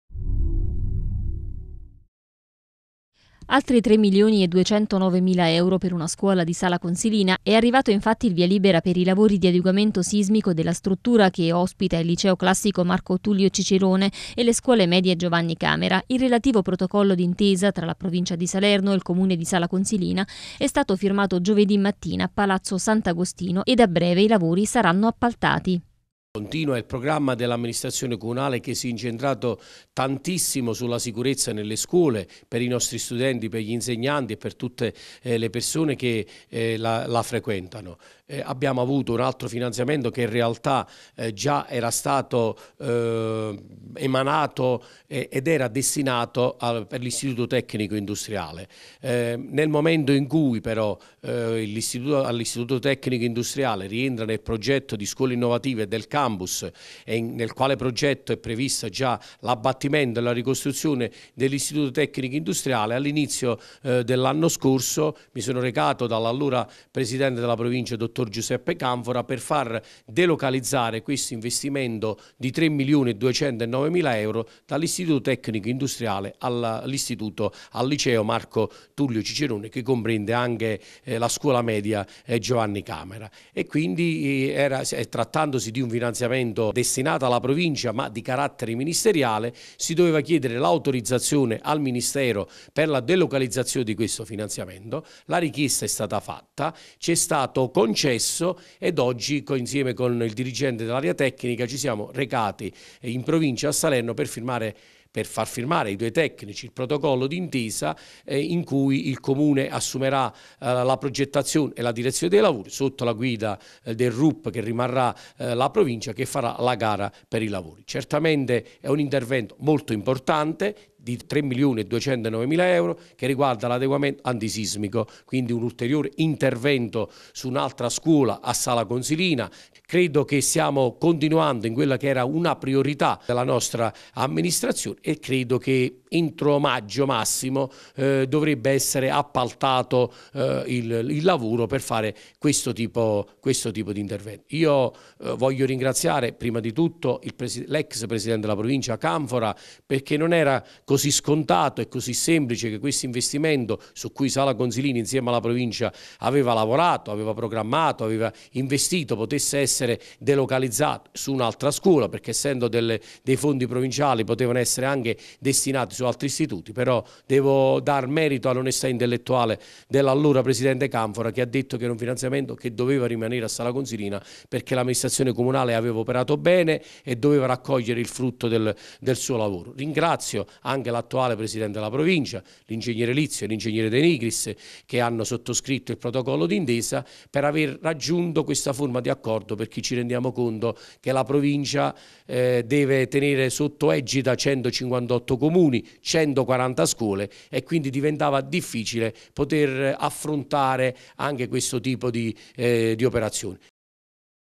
SALA CONSILINA, ADEGUAMENTO SISMICO LICEO CICERONE. VIA LIBERA LAVORI PER 3 MILIONI E 209MILA EURO.
SALA CONSILINA, ADEGUAMENTO SISMICO DEL LICEO CICERONE. VIA LIBERA AI LAVORI PER 3 MILIONI E 209MILA EURO.